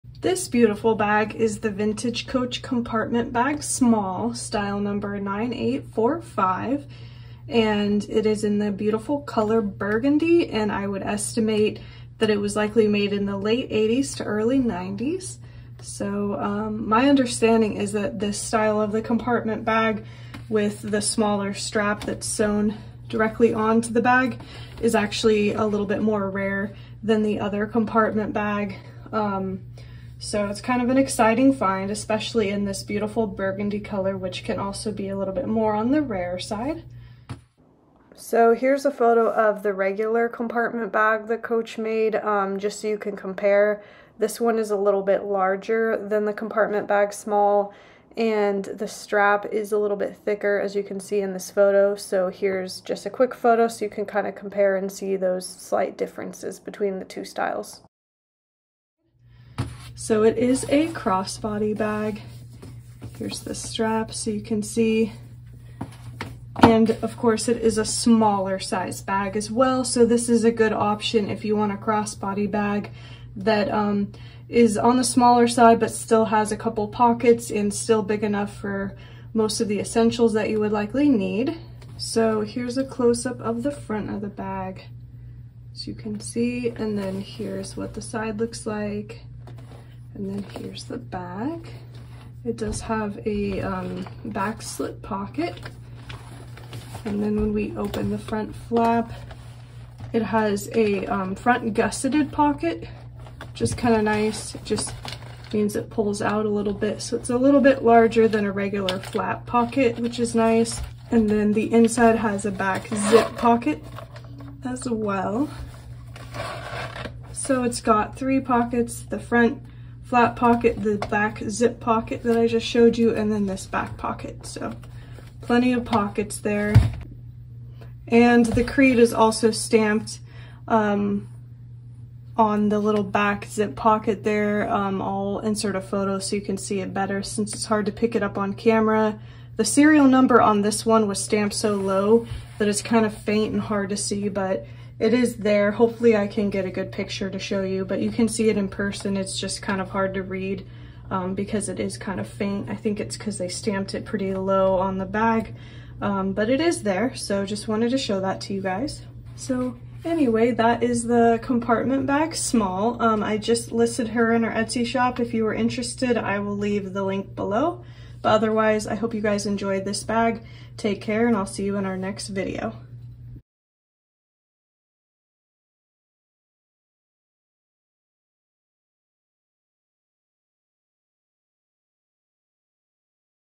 Vintage Coach Bag Tour | Compartment Bag Small | Cristina Moda Vintage Shop
Vintage Coach Bag Tour: Compartment Bag Small

Here's a quick tour of the Vintage Coach Compartment Bag Small!

Item: Cosmetic Compartment Bag Small
Color: Burgundy
Condition: Pre-owned
Style Number: 9845
Era: Late 80's-early 90's (estimate)

This specific item is available for purchase in our Etsy shop!